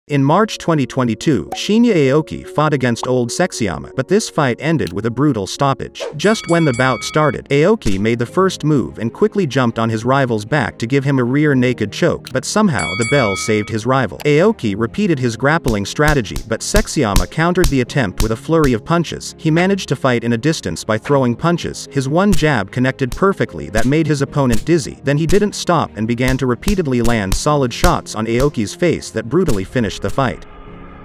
An Intense Rivalry!!😨
Shinya Aoki vs. Yoshihiro Akiyama was an MMA fight in One Championship Fighting Club.

by Florid Miston

Footage Credit : @ONEChampionship